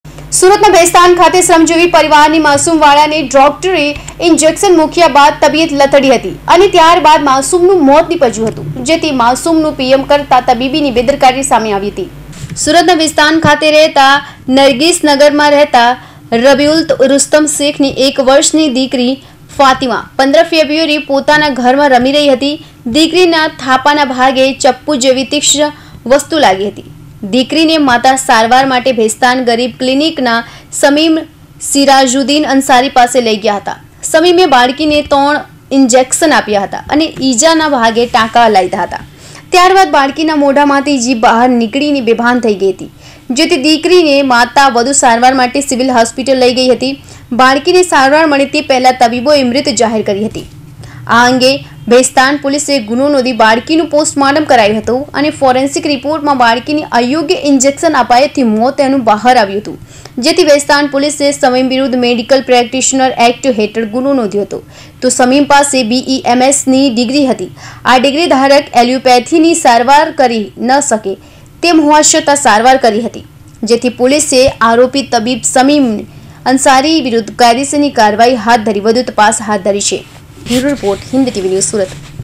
સુરતના ભેસ્તાનમાં તબીબ સામે દાખલ થયો બેદરકારીનો ગુનો,
1 વર્ષની બાળકીને 3 ઈન્જેક્શન આપી દેતાં થયું હતું મોત